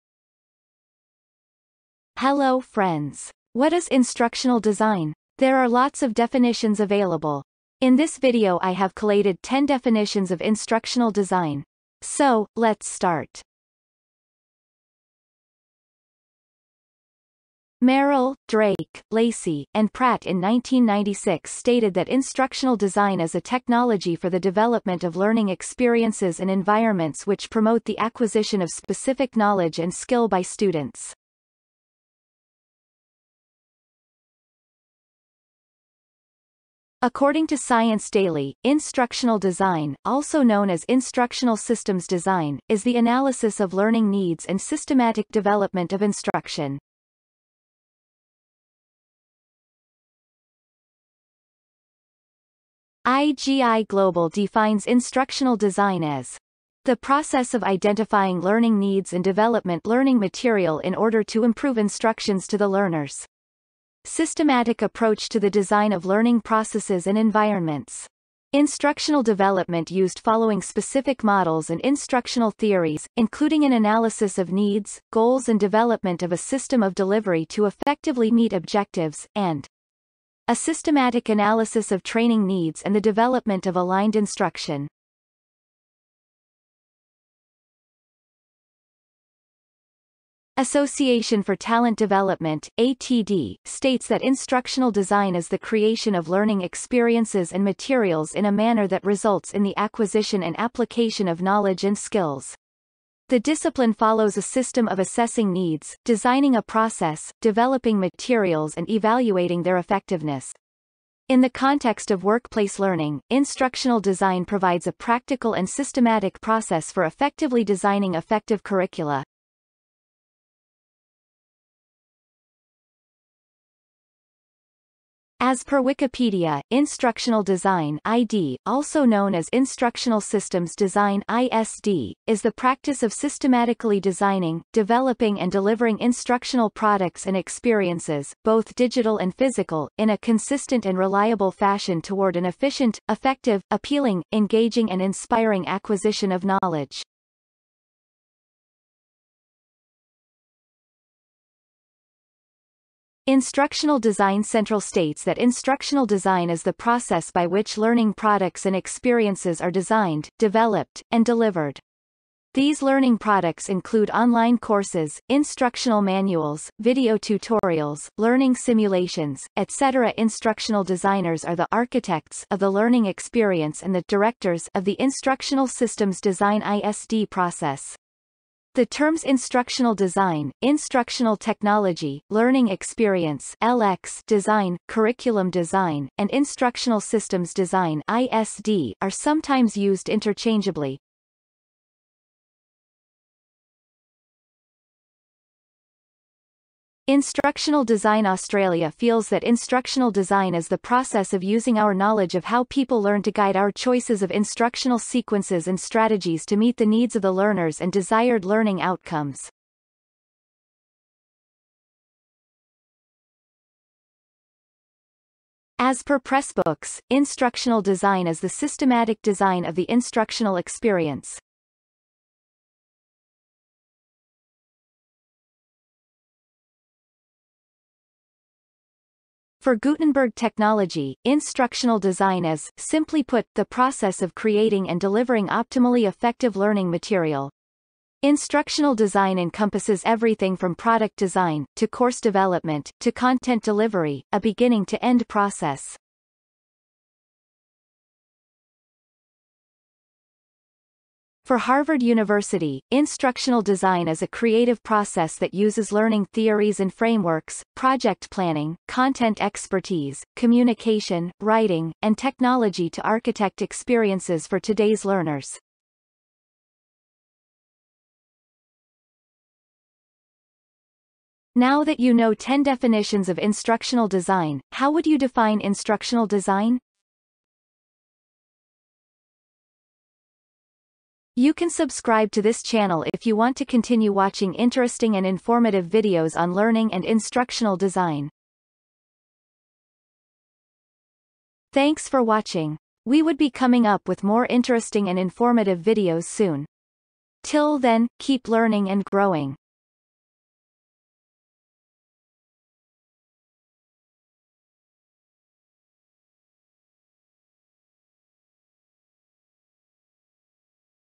What is Instructional Design? - 10 Definitions
What is Instructional Design? This video explains what is instructional design through 10 definitions.

00:00:18 - Definition 1
00:00:37 - Definition 2
00:00:53 - Definition 3
00:01:30 - Definition 4
00:02:04 - Definition 5
00:02:31 - Definition 6
00:03:17 - Definition 7
00:03:35 - Definition 8
00:03:49 - Definition 9
00:04:11 - Definition 10

Links for definitions:
Merrill, M. D.; Drake, L.; Lacy, M. J.; Pratt, J. (1996). “Reclaiming instructional design” (PDF). Educational Technology. 36 (5): 5–7.

Have a look at the other interesting videos.